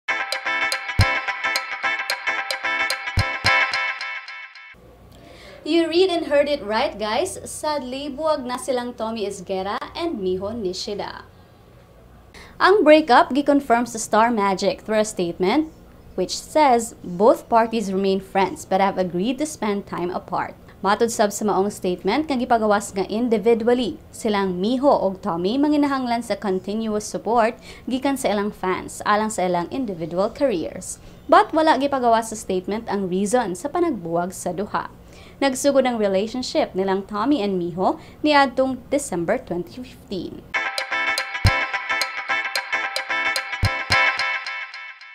Tommy Esguerra, Miho Nishida buwag na
Mga estroya karong adlawa:
1. Weekend power outage schedule sa Veco
2. Stylish summer looks by Cebuano fashion designers
3. Ssang Yong mibalik sa Cebu with European-inspired vehicles

Showbiz:
1. Jake Cuenca ready to race human sa biking accident